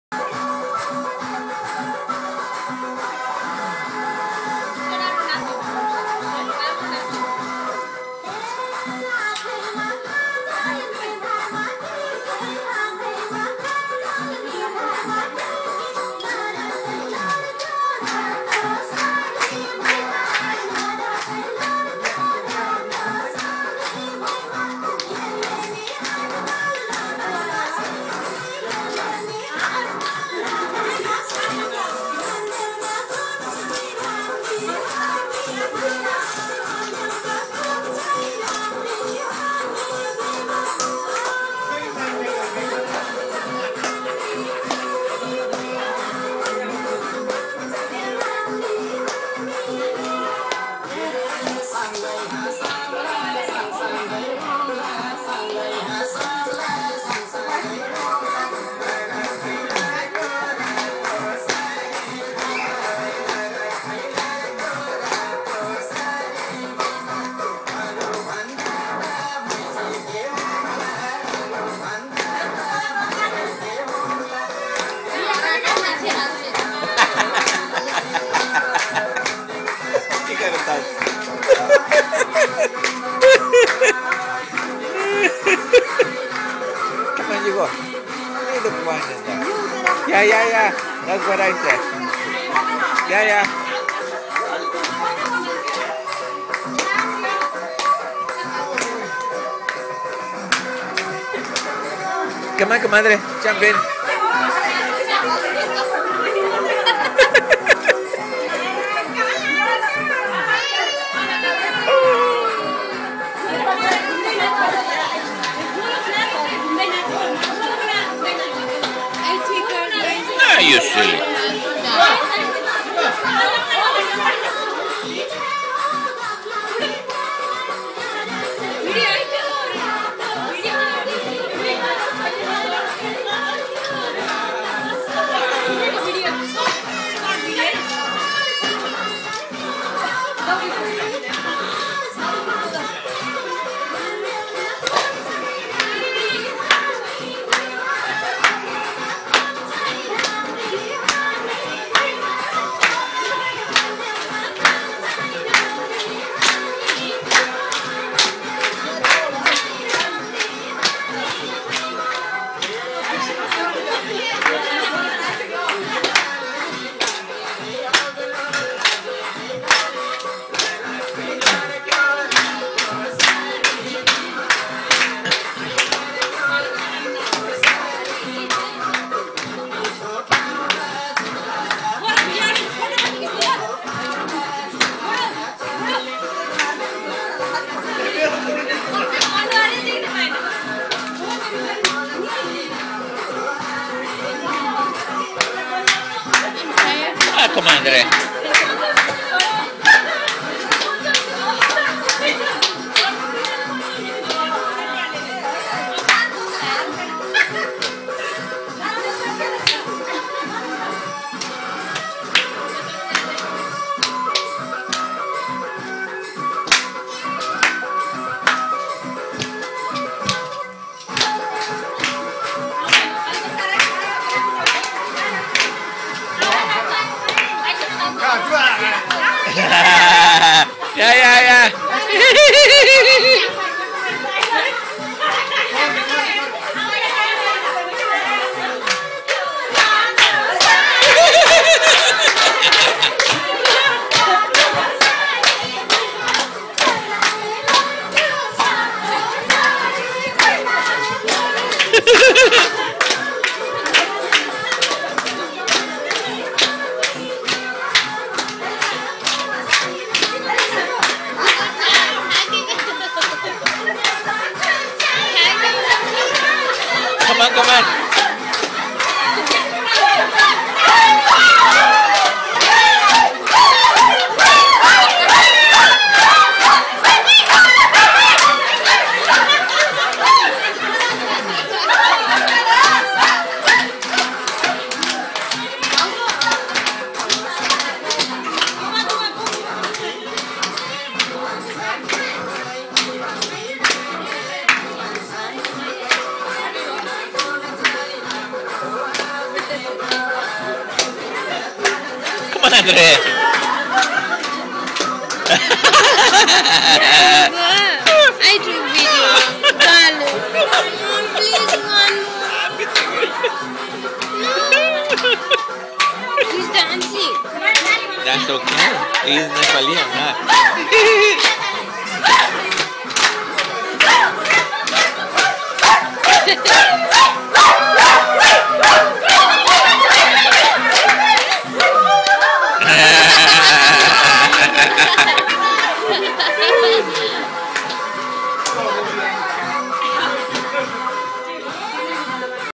Nepali Woman's day
Celebrating Nepali  woman's  day in Victoria Bc Canada